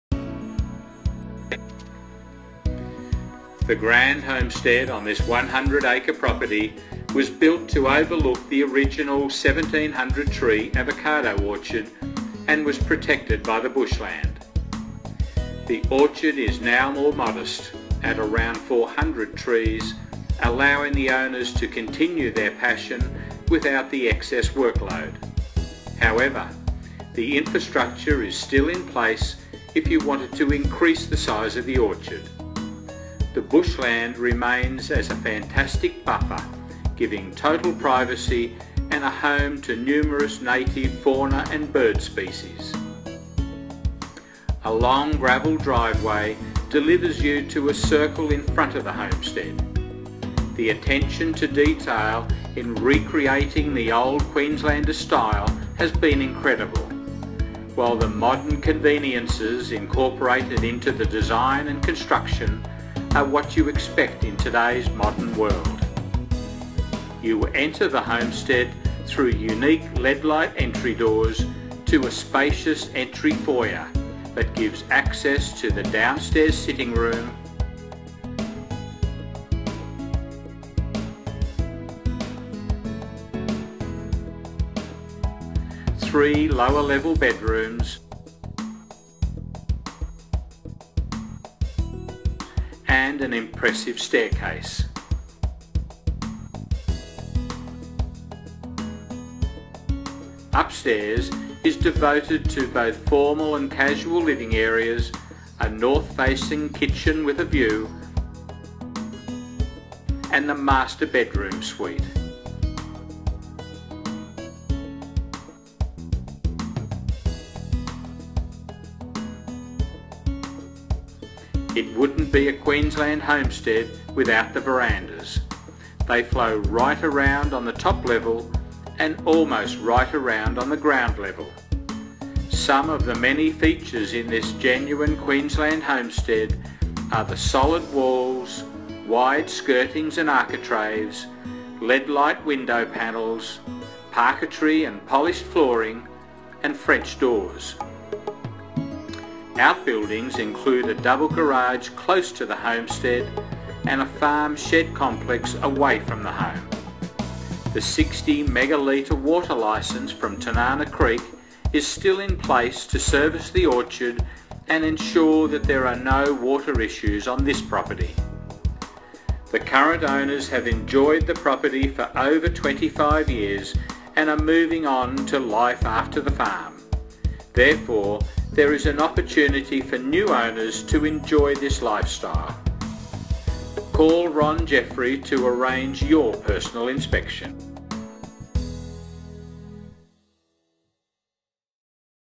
10 MINUTES FROM THE HEART OF MARYBOROUGH!
The grand homestead on this 100 acre property was built to overlook the original 1,700 tree avocado orchard and was protected by the bushland. The orchard is now more modest, at around 400 trees, allowing the owners to continue their passion without the excess workload. However, the infra-structure is still in place if you wanted to increase the size of the orchard. The bushland remains as a fantastic buffer giving total privacy and a home to numerous native fauna and bird species.
A long gravel driveway delivers you to a circle in front of the homestead. The attention to detail in re-creating the “Old Queenslander Style” has been incredible; while the modern conveniences incorporated in the design and construction are what you expect in today’s modern world.
You enter the Homestead through unique leadlight entry doors to a spacious entry foyer that gives access to the downstairs sitting room, 3 lower level bedrooms and an impressive staircase. Upstairs is devoted to both formal and casual living areas, a north-facing kitchen with a view and the master bedroom suite. It wouldn’t be a Queensland Homestead without the verandas. They flow right around on the top level and almost right around on ground level. Some of the many features in this genuine Queensland homestead are the solid walls, wide skirtings and architraves, leadlight window panels, parquetry and polished flooring and French doors.
Outbuildings include a double garage close to the homestead and a farm shed complex away from the home. The 60 mega-litre water licence from Tinana Creek is still in place to service the orchard and ensures that there are no water issues on this property.
The current owners have enjoyed the property for over 25 years and are moving on to life after the farm. Therefore, there is an opportunity for new owners to enjoy this lifestyle.
Call Ron Jeffery to arrange your personal inspection.